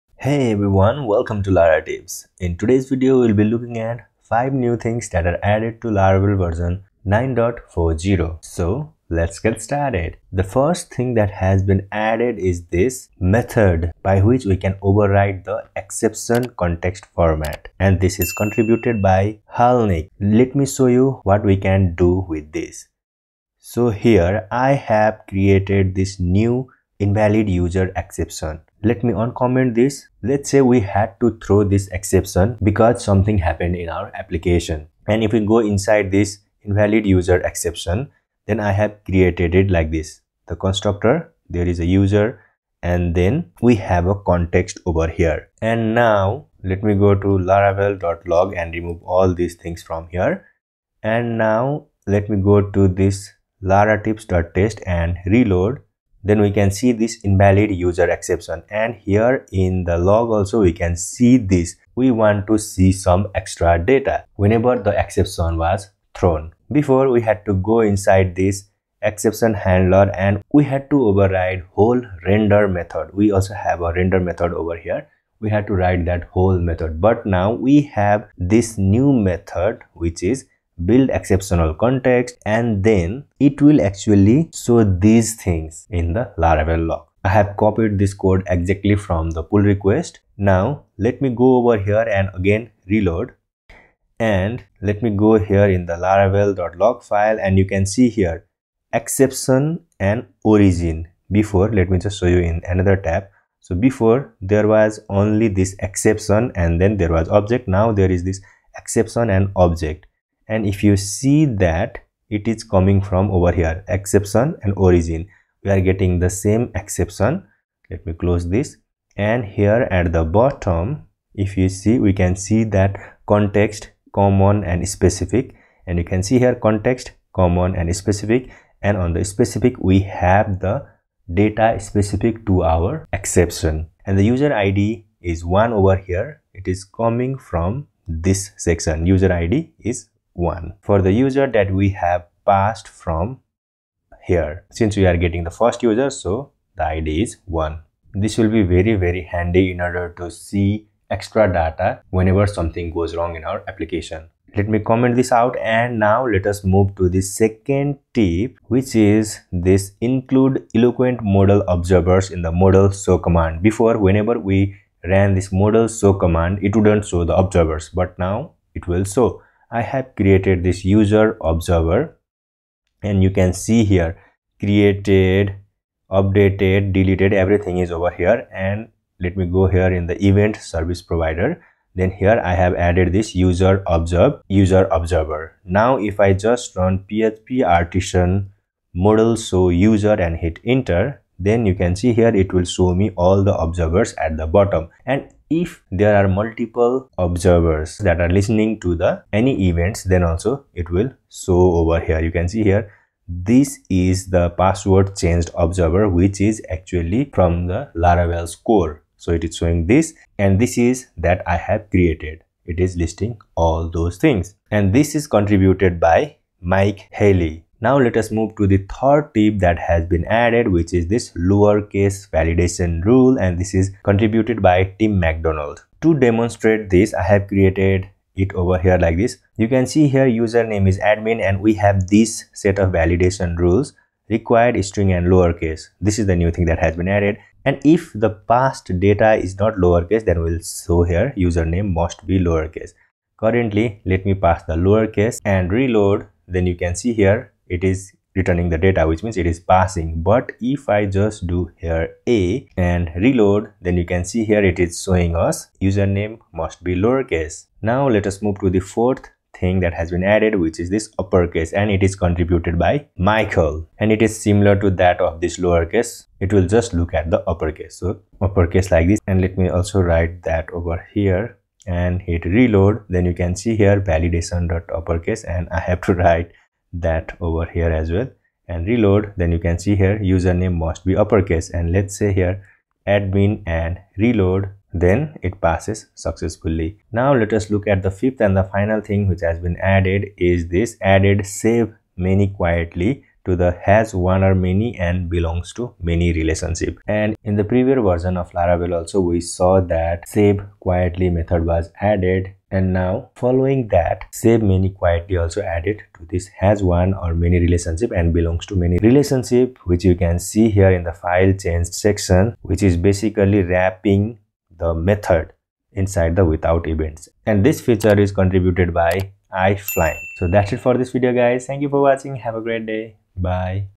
Override Exception Context Format - 5 New Things Added - New In Laravel 9.40
Here, we will be looking at 5 new things that are added to the latest version of Laravel which is v9.40

=== OFFER ===

00:00 Intro

00:10 Add method to be able to override the exception context format

02:34 Include Eloquent Model Observers in model:show command

03:37 Add lowercase validation rule

04:17 Add uppercase validation rule

04:46 Added saveManyQuietly to the hasOneOrMany and belongsToMany relations

=== Theme, Font and Extensions I use ===

=== Attribution ===
Boy waving hand Video by VideoPlasty .com from Pixabay